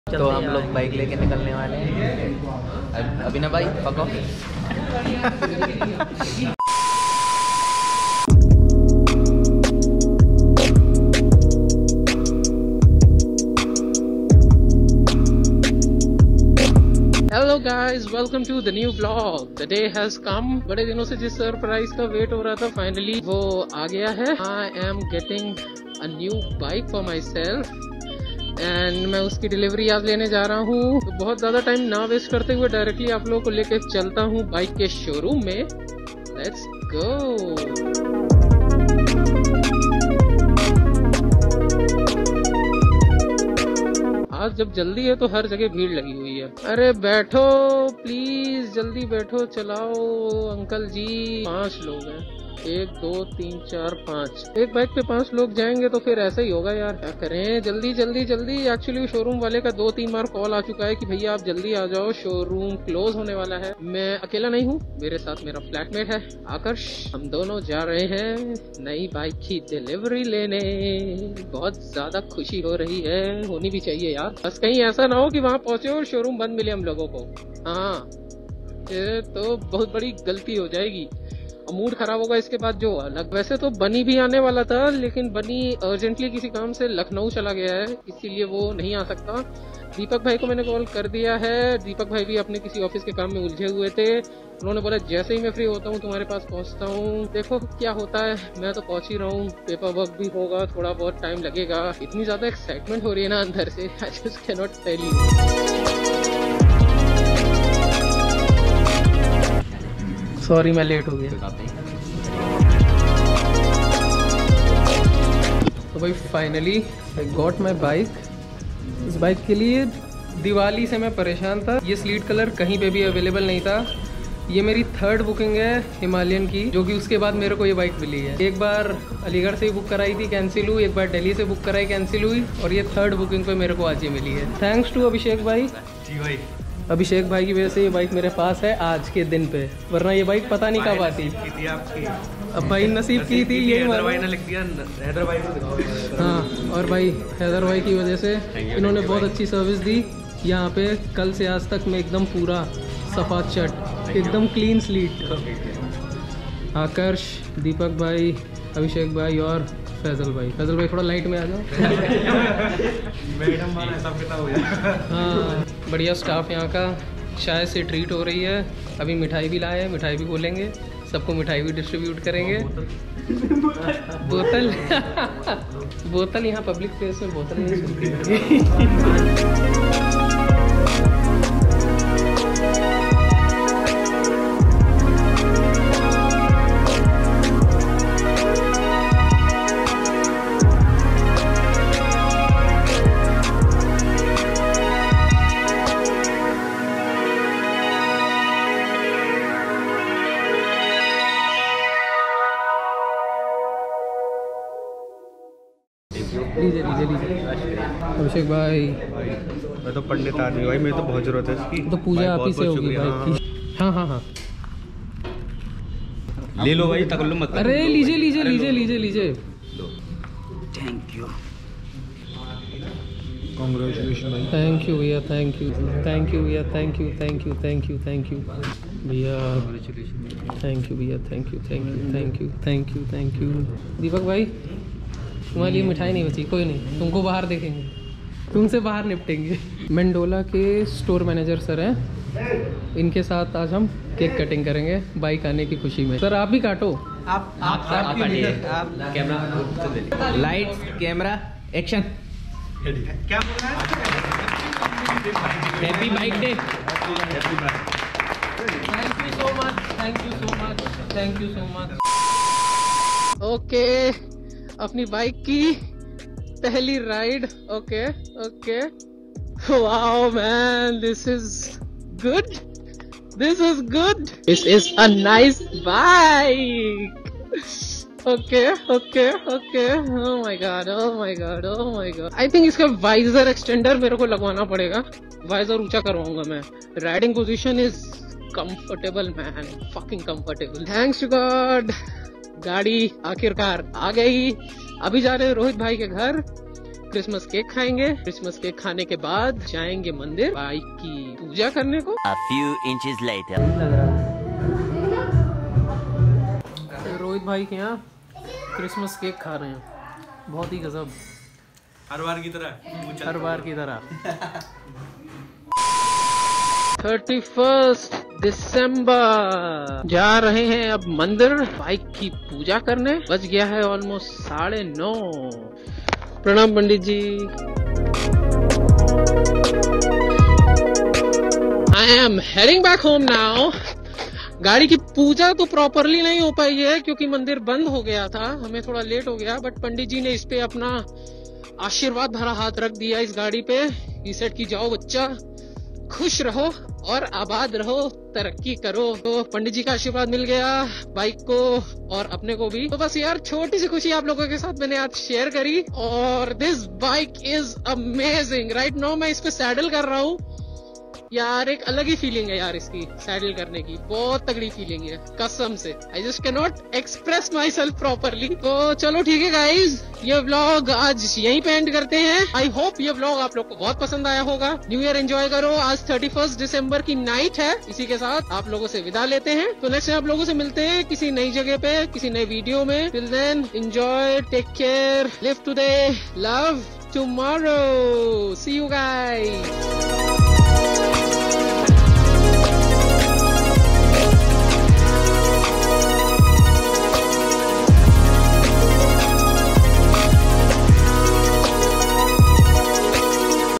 DREAM BIKE || ROYAL ENFIELD HIMALAYAN || HAPPY NEW YEAR (1ST VLOG IN 2021)
FINALLY GOT MY NEW BIKE, HAPPY AF... BEST WAY TO END THE YEAR. THANX A LOT FOR FRIENDS AND FAMILY FOR SUPPORT.
MAKING FILMS/FLICKS/VIDEOS REALLY MEANS A LOT TO ME.
HAVE BIG PLANS FOR THIS YEAR, WE SHALL DO IT TOGETHER